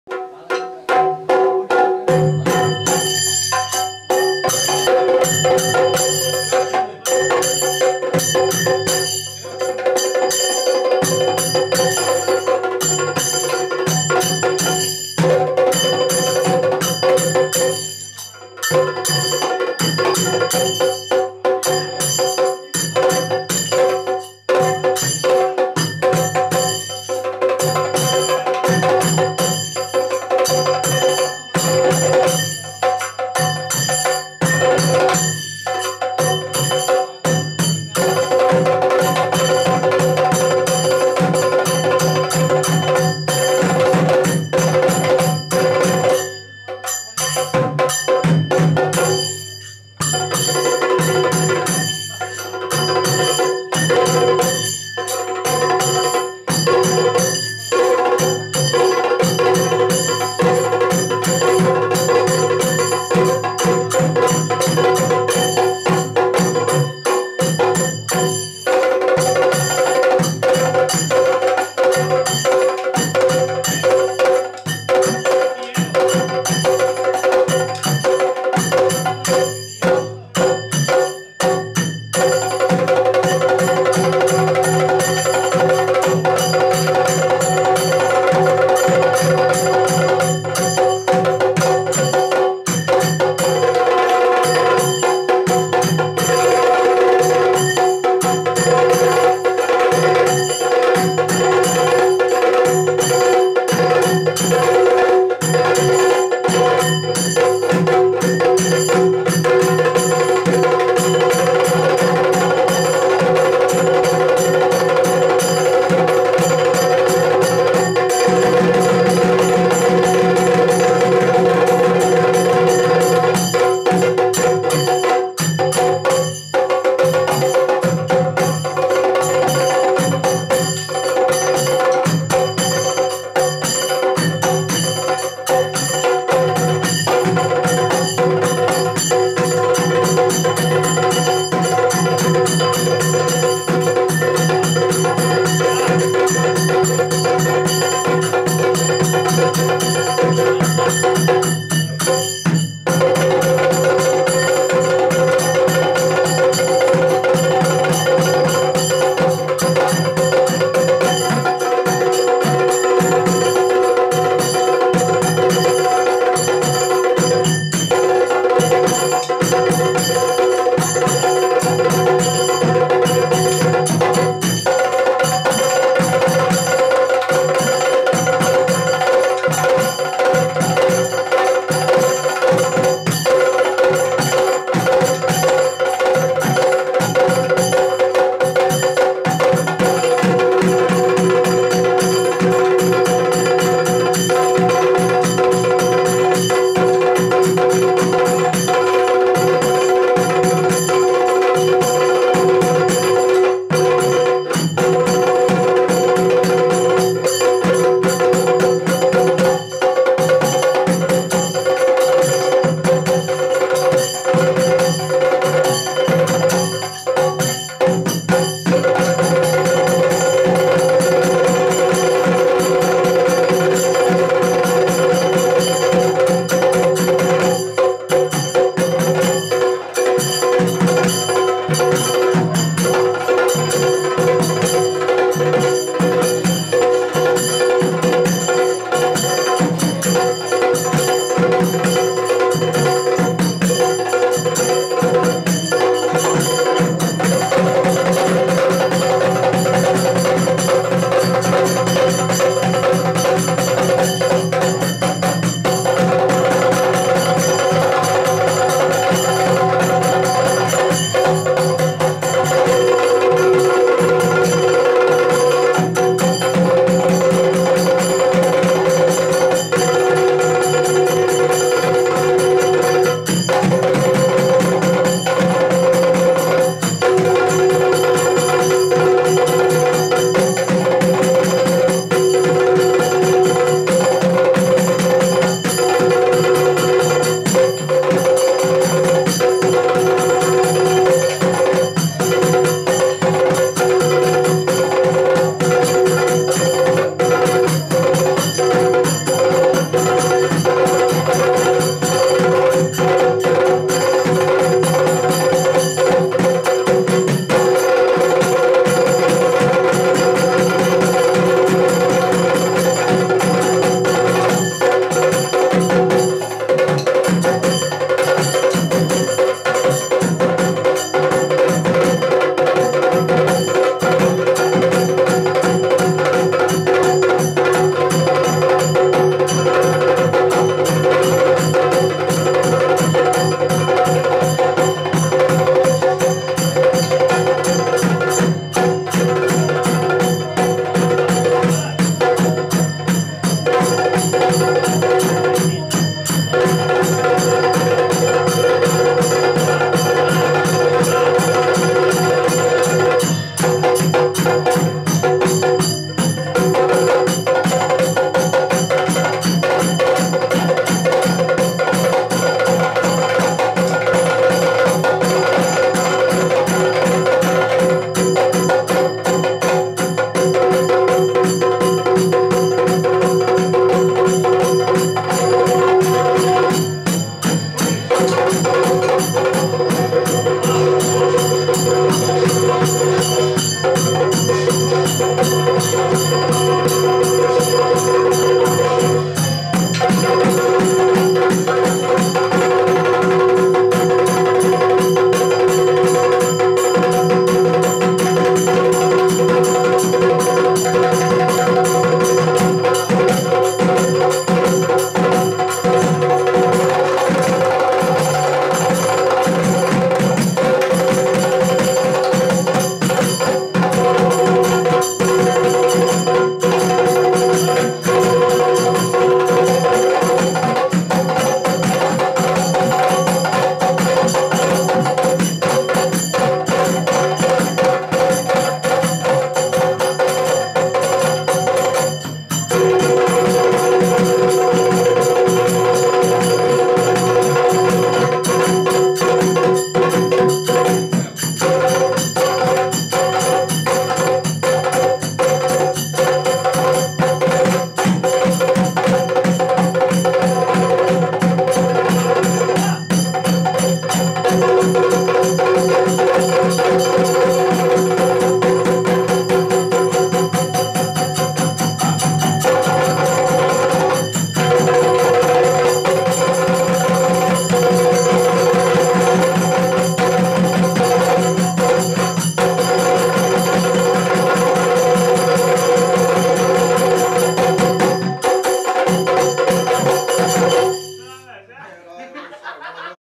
೫ ಚೆಂಡೆ ವಾದಕರು ೨ ಚೆಂಡೆಯನ್ನು ನುಡಿಸಿದ ಪರಿ ಅದ್ಭುತ | 5Chende Artist Played With 2Chende was unbelievable
SuhasBadhya Please
👀Watch #

Thanking you

Suhas Badhya